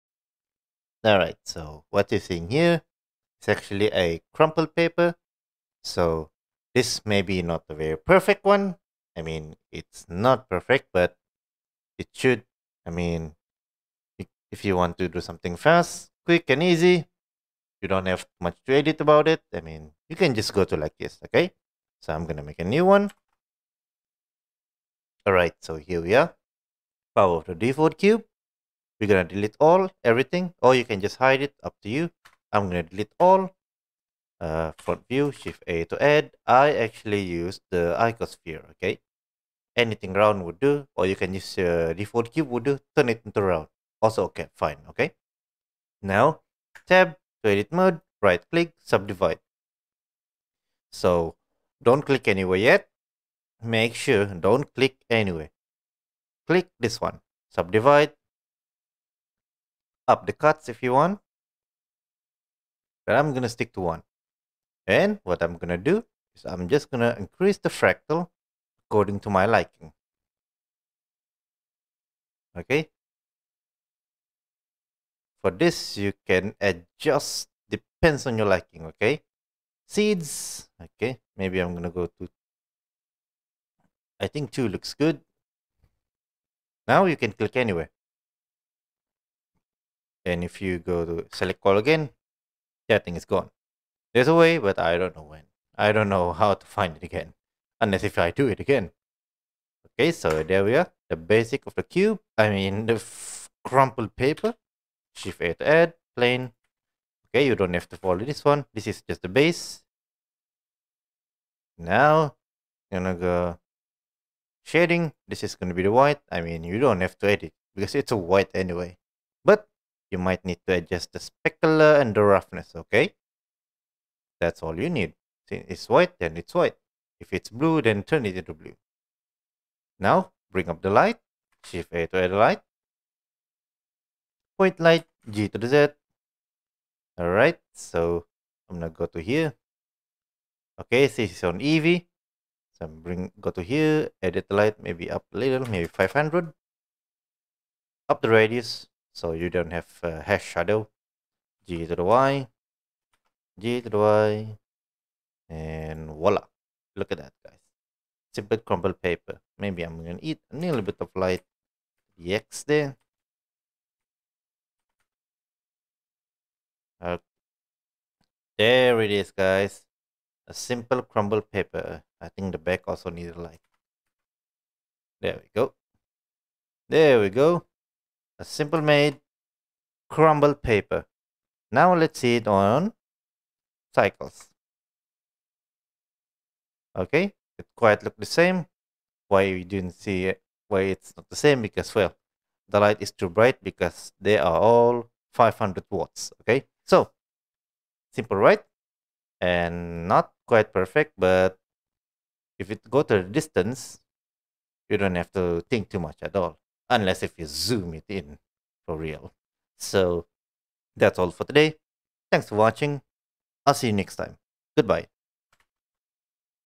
Blender Tutorial | Crumple Paper
Simple and quick version of crumple paper, consider this as shortcuts if you don't wanna follow the proper way or maybe you in such a hurry. This is not perfect, but if you rushing, this might be solution for last minute work!